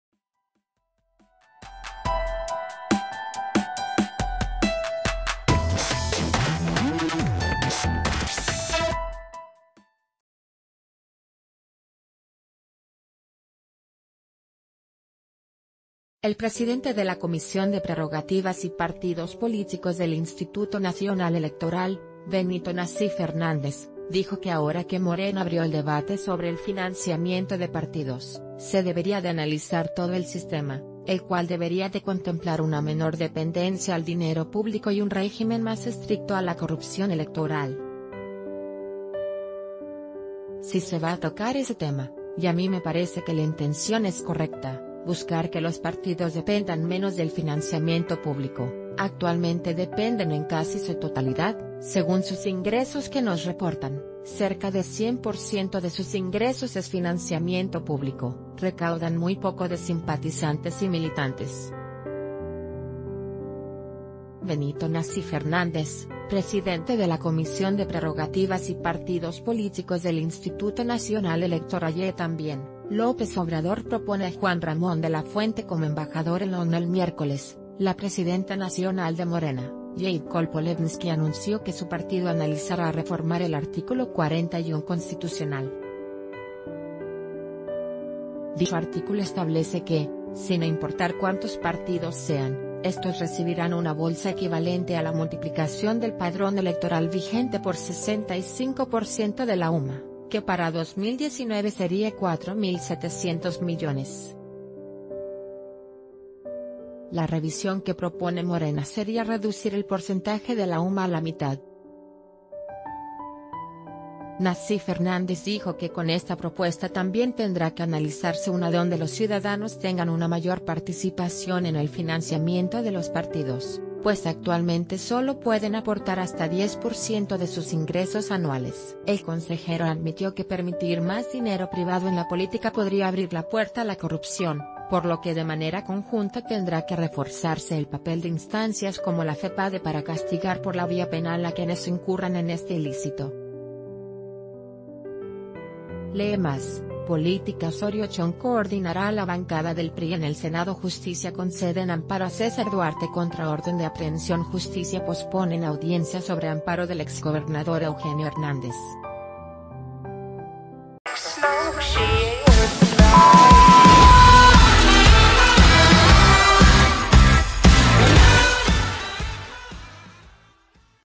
Intención de Morena es correcta, partidos deben depender menos de financiamiento público: Nacif
El presidente de la Comisión de Prerrogativas y Partidos Políticos del Instituto Nacional Electoral, Benito Nacif Hernández, dijo que ahora que Morena abrió el debate sobre el financiamiento de partidos, se debería de analizar todo el sistema, el cual debería de contemplar una menor dependencia al d...